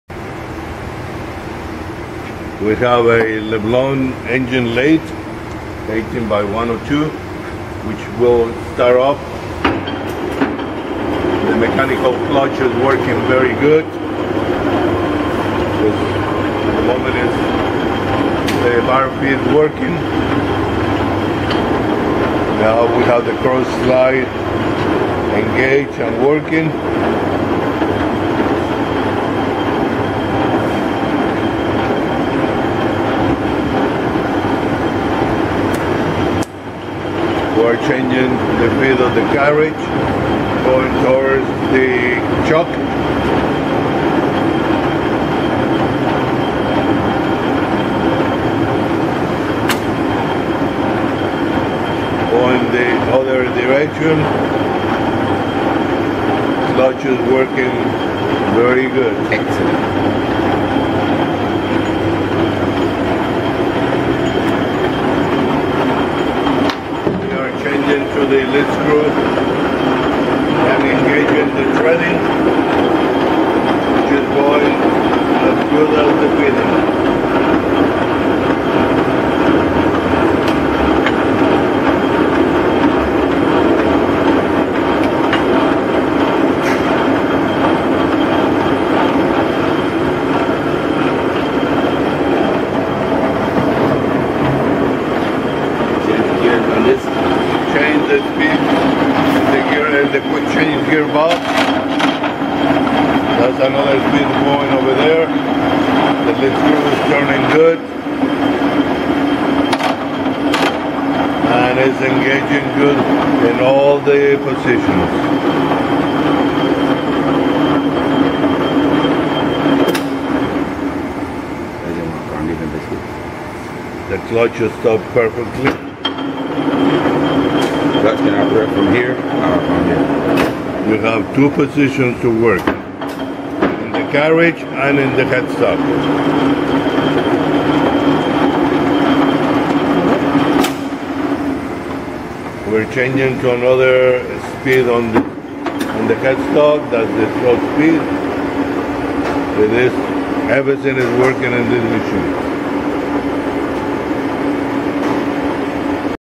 LeBlond 18" x 102" Lathe
This machine is as quiet as a mouse. Turns on without any issues. Has a motor of 10 hp, 440v. This lathe also has a large spindle at 2".